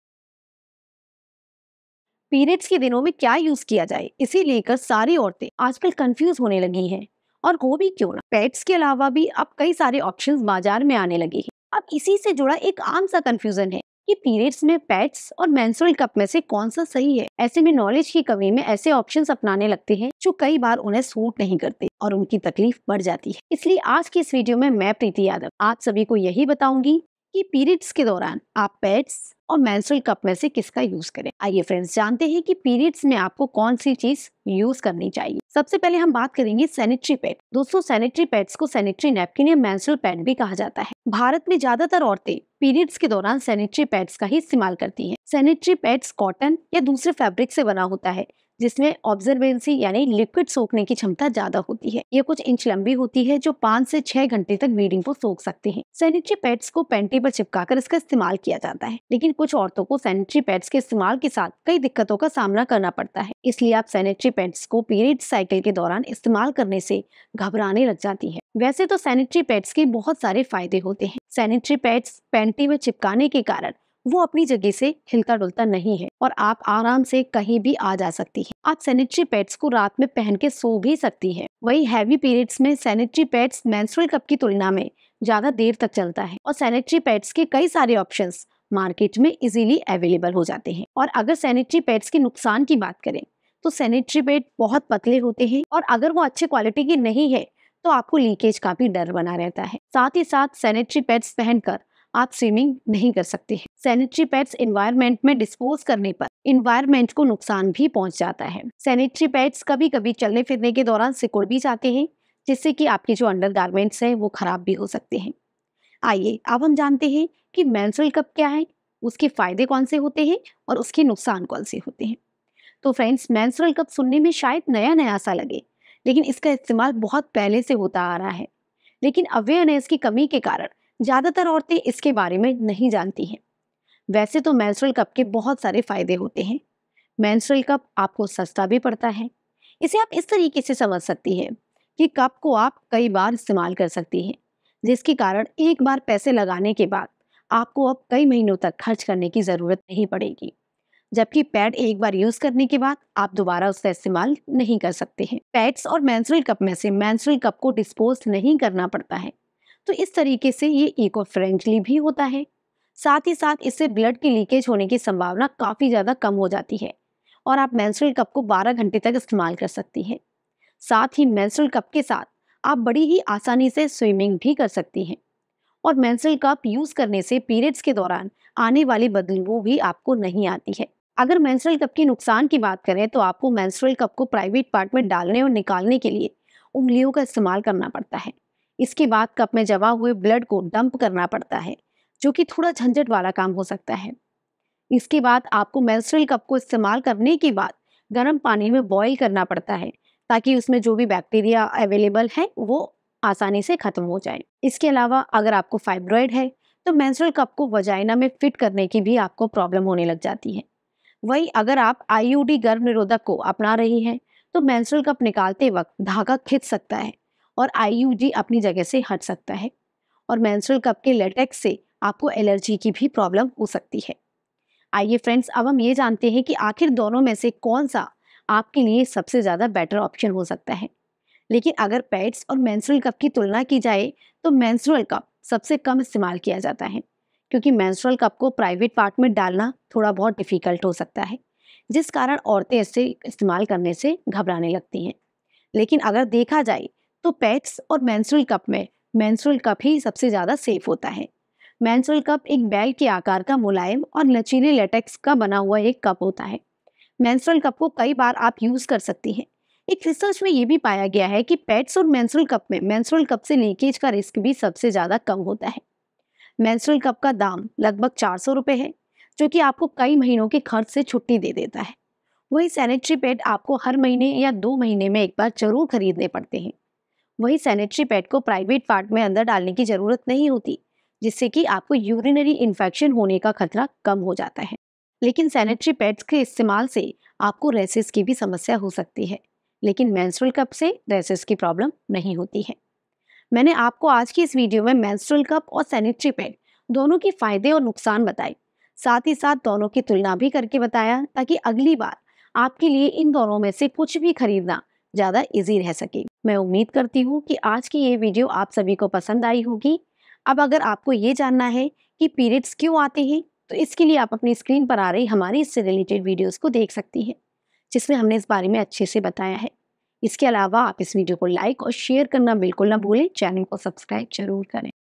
Menstrual Cup vs Sanitary Pads in Hindi | What is Menstrual Cup and How to use it in Hindi
पीरियड के दिनों में क्या use किया जाए इसे लेकर सारी औरते आजकल कंफ्यूज होने लगी हैं और हों भी क्यों न? पैड्स के अलावा भी अब कई options बाजार में आने जो लगे हैं। अब इसी से जुड़ा एक आम कंफ्यूजन है कि पीरियड्स में पैड्स और मेंस्ट्रुअल कप में से कौन सा सही है? ऐसे में knowledge की कमी में ऐसे options अपनाने लगती हैं, जो कई बार उन्हें सूट नहीं करते और उनकी तकलीफ बढ़ जाती है। इसलिए आज की वीडियो में मैं प्रीति यादव आपको बताऊंगी की पीरियड्स में आप पैड्स, टैम्पून और मेंस्ट्रुअल कप में से किसका यूज करें।